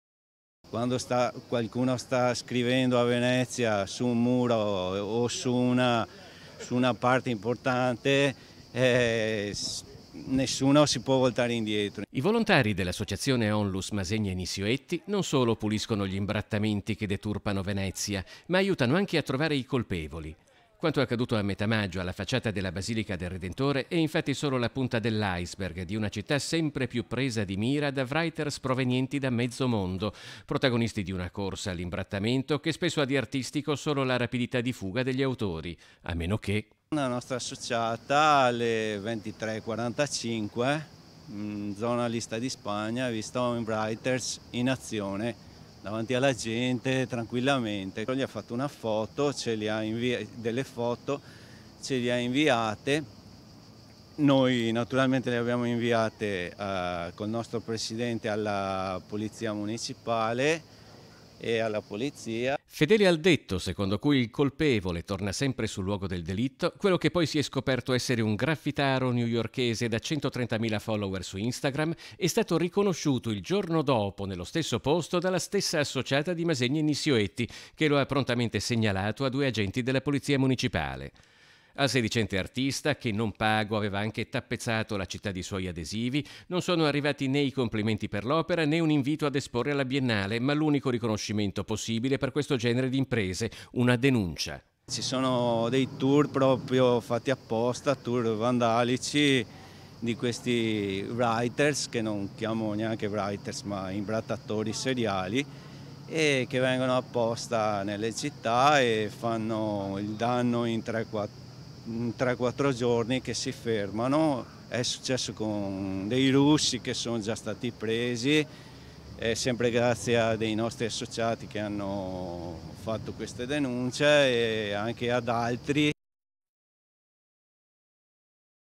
DA NEW YORK A VENEZIA PER IMBRATTARE MURI: DENUNCIATO | 13/07/2022
A3 NEWS Venezia 13/07/2022 - VENEZIA - Denunciato grazie all'associazione «Masegni & Nizioleti» un writer newyorchese sorpreso ad imbrattare diversi muri di Venezia. Sono sempre più gli episodi del genere con protagonisti stranieri.  || I volontari dell’associazione onlus “Masegni e Nizioleti” non solo puliscono gli imbrattamenti che deturpano Venezia, ma aiutano anche a trovare i colpevoli. Quanto accaduto a metà Maggio alla facciata della Basilica del Redentore, è infatti solo la punta dell’iceberg di una città sempre più presa di mira da writers provenienti da mezzo mondo, protagonisti di una corsa all’imbrattamento che spesso ha di artistico solo la rapidità di fuga degli autori.Fedele al detto secondo cui il colpevole torna sempre sul luogo del delitto, quello che poi si è scoperto essere un graffitaro newyorkese da 130mila follower su Instagram è stato riconosciuto il giorno dopo nello stesso posto dalla stessa associata di “Masegni e Nizioleti” che lo ha prontamente segnalato a due agenti della Polizia Municipale. Al sedicente artista, che non pago aveva anche tappezzato la città di suoi adesivi,non sonoarrivati né i complimenti per l’opera, né un invito ad esporre alla Biennale, ma l’unico riconoscimento possibile per questo genere di imprese: una denuncia. - Intervistati ALBERTO ALBERTI (ASSOCIAZIONE “MASEGNI & NIZIOLETI” ONLUS) (Servizio di Filippo Fois) - Segui Antenna Tre anche sul digitale terrestre! Visita il sito